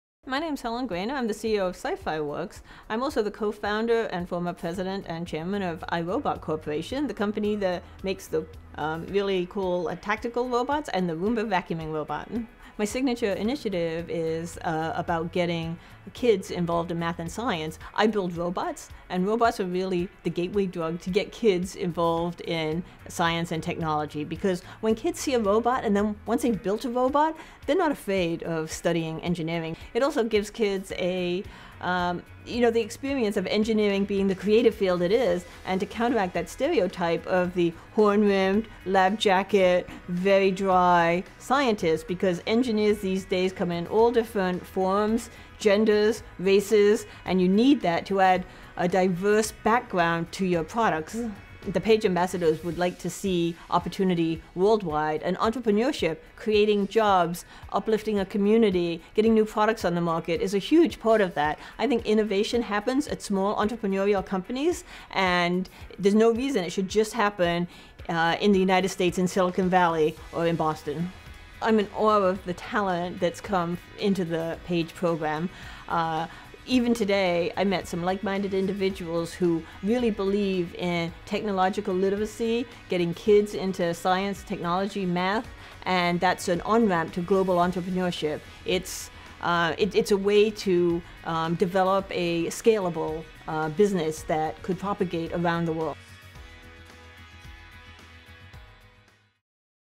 PAGE Ambassador – Helen Greiner of CyPhy Works
Presidential Ambassador for Global Entrepreneurship (PAGE) and CEO of CyPhy Works Helen Greiner speaks about her Signature Initiative, which aims to inspire kids to pursue careers in the math and engineering fields.

PAGE is a group of America’s most respected business creators who have committed to sharing their time, energy, ideas, and experience to help develop the next generation of entrepreneurs around the world. U.S. Secretary of Commerce Penny Pritzker chairs the PAGE initiative.